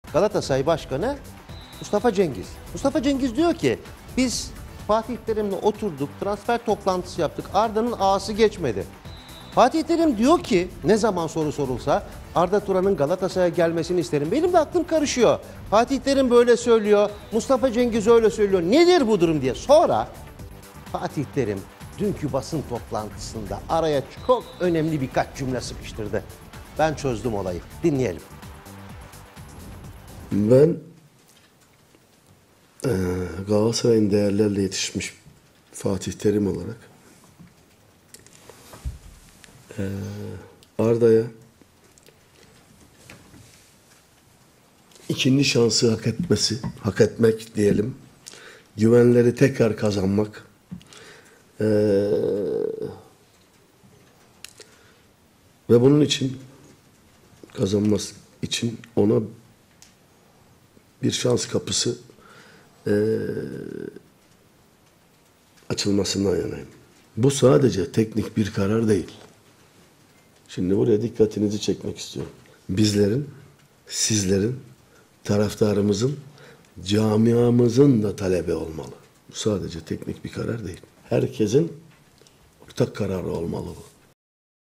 Fatih Terim: "Arda'yı Camianın İstemesi Gerekiyor"
Fatih Terim Arda Turan'ı istiyor mu? Evet, Peki Başkan Mustafa Cengiz; biz Fatih Terim'le oturduk Arda'nın a'sı geçmedi dedi mi? Dedi. Sayın seyirciler.. Fatih Terim'in bir cümlesi durumu çok netleştirdi. Fatih Terim diyor ki "evet ben istiyorum Arda'yı. Ama benim istemem yetmez. Bu sadece bir teknik karar değil"